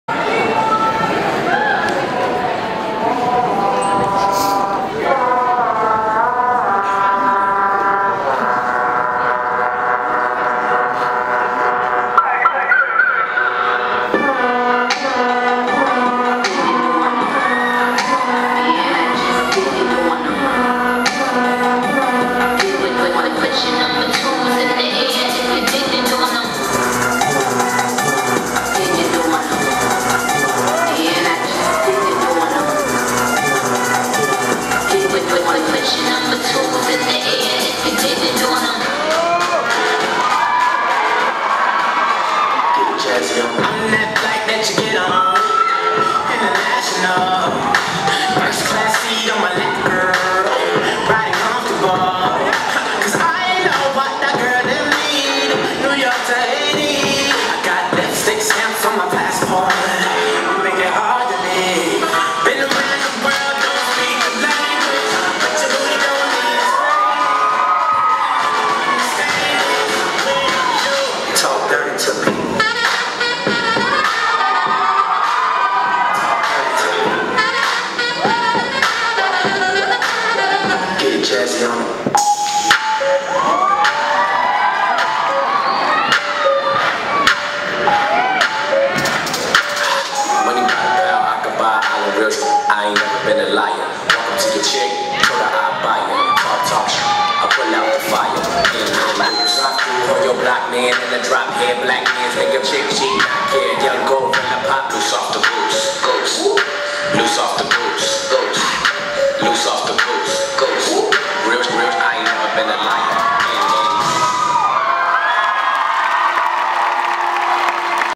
SMHS Dance Team @ SMHS WINTER RALLY 2013
Dance Team | (WATCH in 1080pHD)

The Dance Team performs in rallies at school, halftime shows during football and basketball games, and an annual dance concert in May that is run by the SMHS Dance Program Director, Robyn Tribuzi. Lastly, this team competes locally in competitions around the bay area. This team focuses mostly on a hip-hop style dance, but will venture outside and explore some other styles that fuse hip-hop as well.